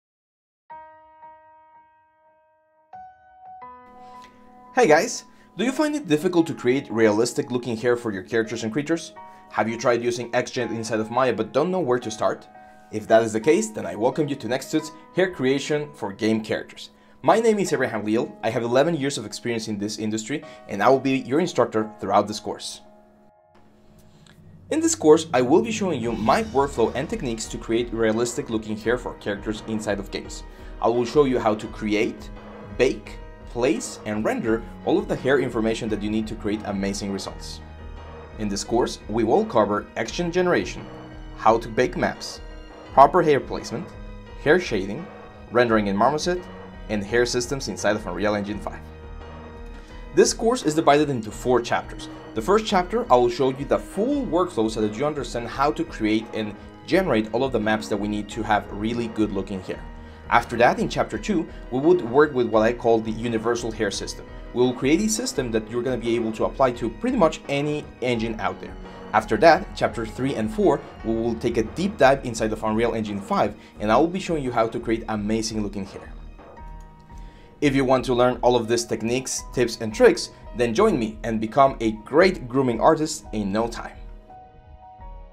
Hair Creation for Game Characters | Course Promo
Hey Guys, Do you find it difficult to create real time hair for your characters and creatures?
 Have you tried using Xgen inside of Maya but don’t know where to start? If that is the case then I welcome you to Hair Creation for Game Characters.

WHY SHOULD YOU LEARN FROM ME:

My name is Abraham Leal and I have 11 years in the industry, throughout this course I will show you my technique and workflows to create amazing hair for your portfolio.

BENEFITS :

In this course we will cover all of the necessary information to generate, bake, place and render hair for your characters. We will take a deep dive in the how and why we do what we do to obtain this amazing results.

you'll be able to create hair cards for real time characters

You will be able to use Xgen to create realistic Grooms

WHAT WILL I LEARN:

Xgen Hair Generation

Creating Hair Maps

Proper Hair Placement

Hair Shading

Rendering in Marmoset

Hair Systems in Unreal Engine 5

Making the eyelashes with tubes

IS THIS COURSE RIGHT FOR ME:

I have designed this course for intermediate 3d modelling students, who want to create realistic Hair for their characters and creatures. A basic Understanding of Maya, Photoshop and Arnold Renderer is expected.

The course is also for artists who want to learn about the Xgen Addon and how to achieve realistic looking hair.

WHO IS NOT THE IDEAL STUDENT:

This course is not designed for absolute beginners in Maya.

WHAT SHOULD I KNOW OR HAVE FOR THE COURSE:

Basic Knowledge of Maya, Photoshop and Arnold

You should have Maya, Photoshop, Marmoset Toolbag and Unreal Engine.

JOIN ME NOW:

If you want to learn all of this techniques then join me and be ready to become a great Grooming Artist